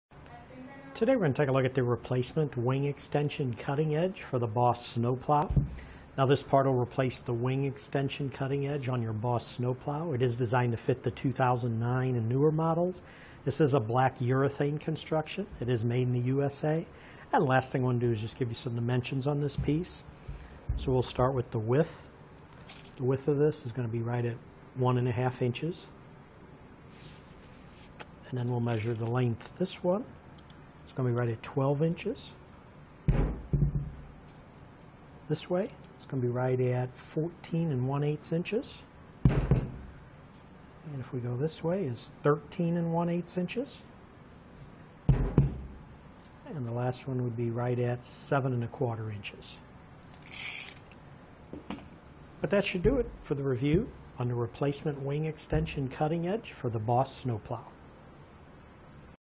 etrailer | Replacement Cutting Edge Wing Extension for Boss Snow Plow Feature Review
Today, we're going to take a look at the replacement wing extension cutting edge for the Boss snow plow. Now this part will replace the wing extension cutting edge on your Boss snow plow. It is designed to fit the 2009 and newer models. This is a black urethane construction. It is made in the USA. And last thing I want to do is just give you some dimensions on this piece.

So we'll start with the width. The width of this is going to be right at one and a half inches, and then we'll measure the length. This one, it's going to be ready at 12 inches. This way it's going to be right at 14 and one eighths inches. Ane, if we go this way it's 13 and one eighths inches.

And the last one would be right at seven and a quarter inches. But that should do it for the review on the replacement wing extension cutting edge for the Boss snowplow.